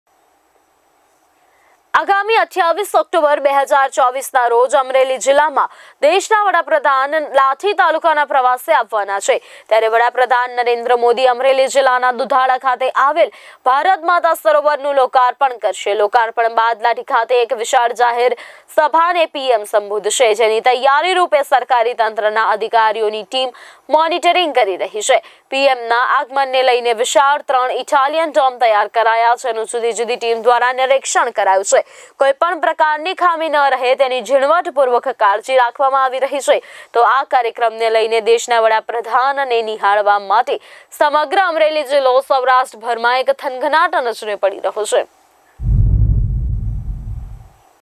વડાપ્રધાન નરેન્દ્ર મોદી આવશે અમરેલીના પ્રવાસે
દુધાળા ખાતે આવેલા ભારત માતા સરોવરનું કરશે લોકાર્પણ
PM લોકાર્પણ બાદ લાઠી ખાતે વિશાળ જાહેરસભાને સંબોધશે

GTPL No. 274 | Vodafone TV | Paytm | Daily Hunt | Jio T0V | Joy TV | Rock TV | Harsh Communication | Siti Cable no. 576 |

Reporting By VR TEAM